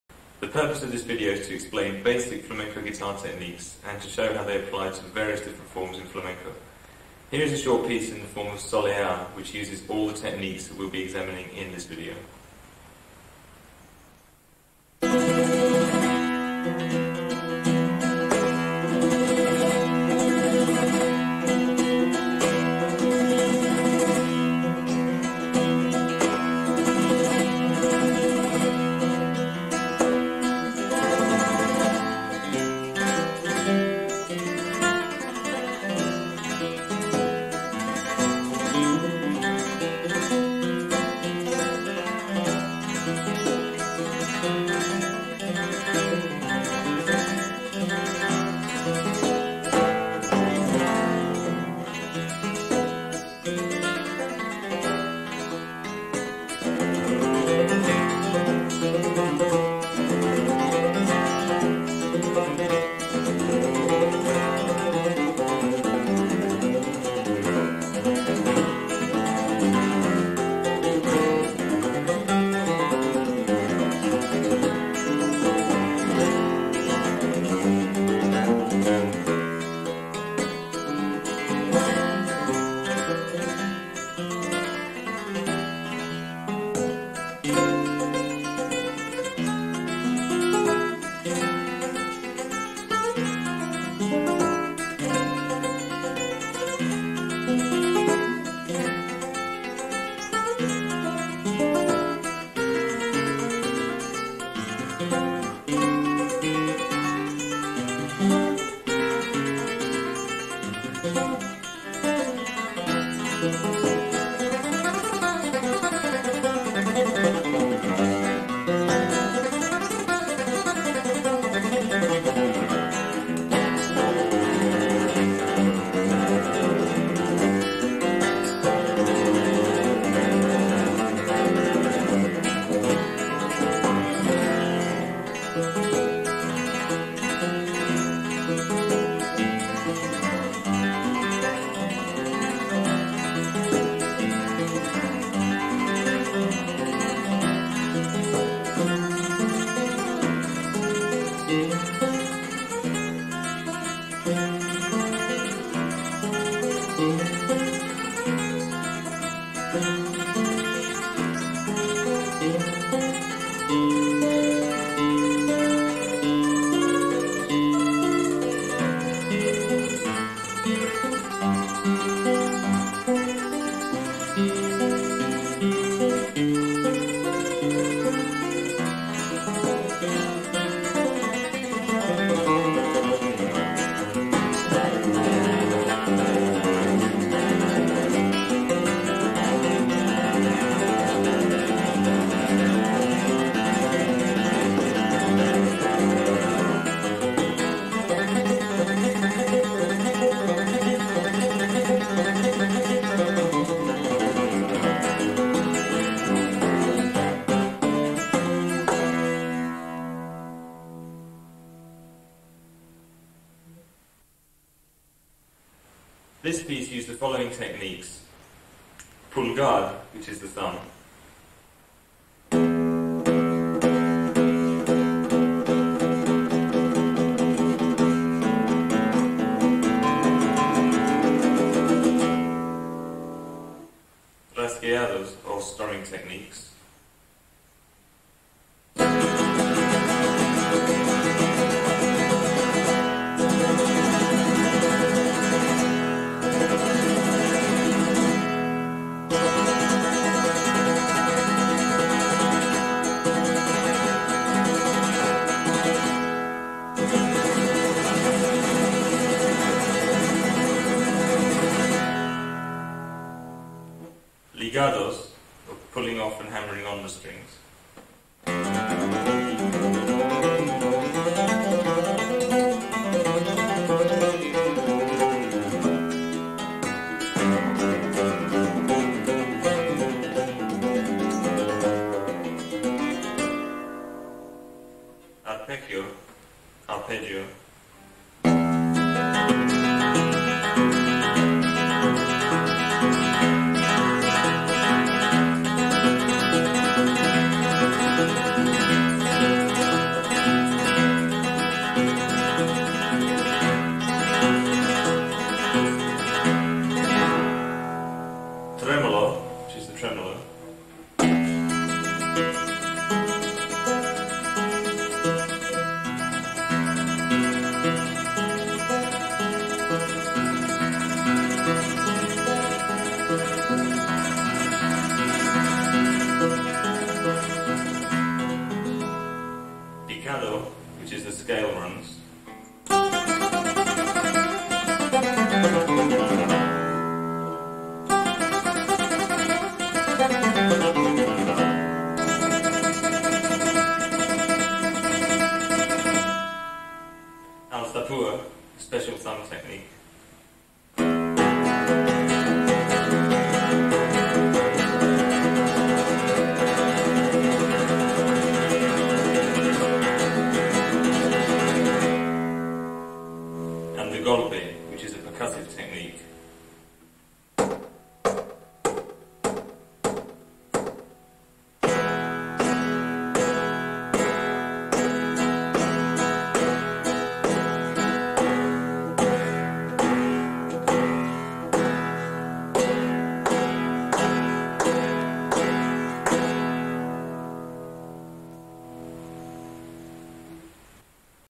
Flamenco Guitar Techniques Pt.2 - Solea and Technique Demo (1996 VHS Video by Francisco Antonio)
for more flamenco guitar content.

This video contains the opening Solea demo plus introduction to techniques from my 1996 VHS Video release "Flamenco Guitar Techniques and Demonstrations."

To see a PDF with full video sleeve, advertising flyer, explanatory booklet, announcement of launch at the Spanish Guitar Centre, London, and a review from Classical Guitar magazine, go to the following link: